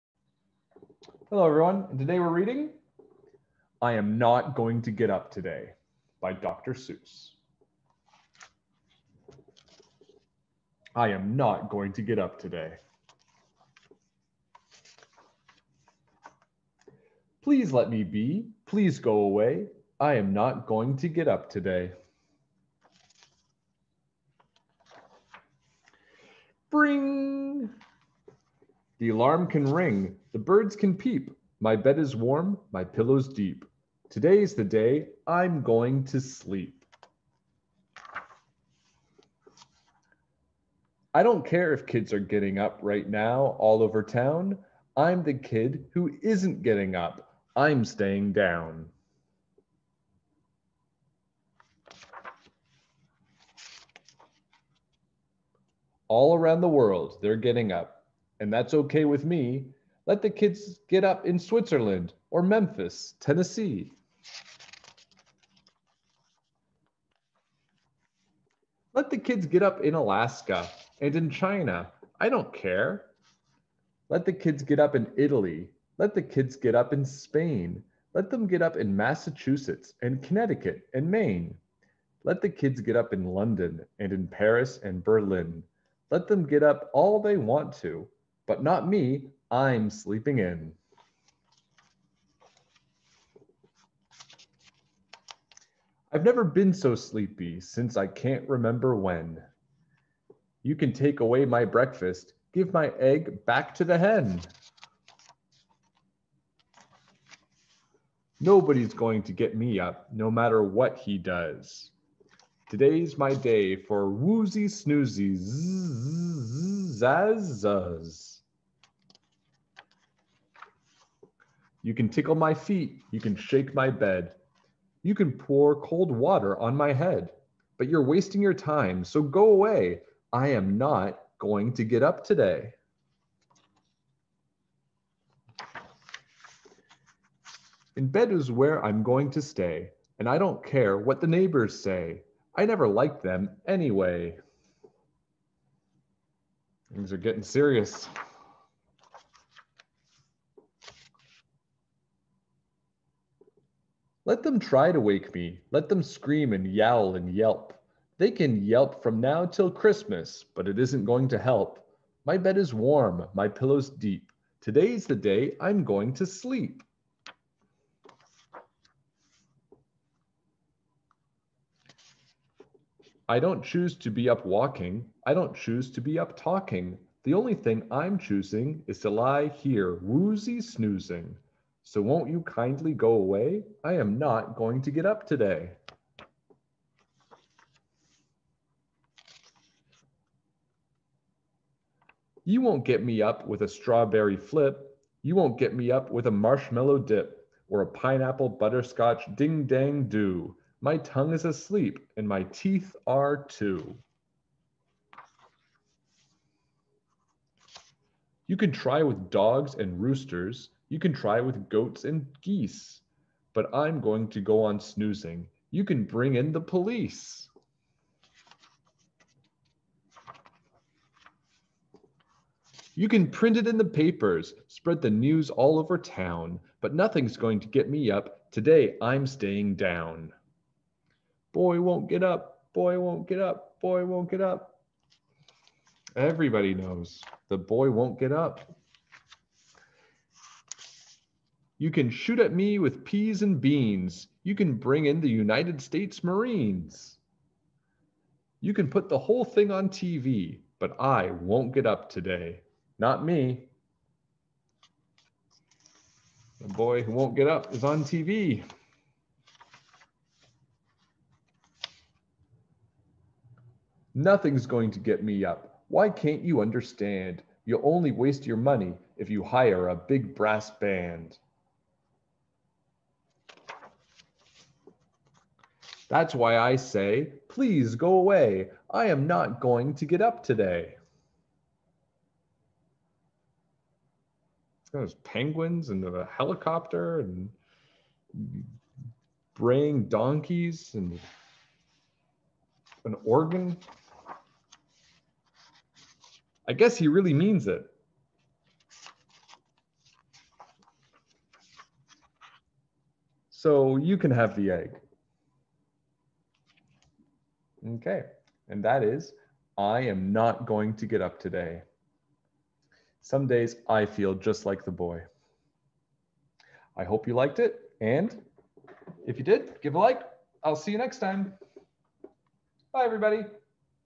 I Am Not Going to Get up Today! - Mr. Hogg Reads
Here I read I Am Not Going to Get up Today, a book by Dr. Seuss. Illustrated by James Stevenson.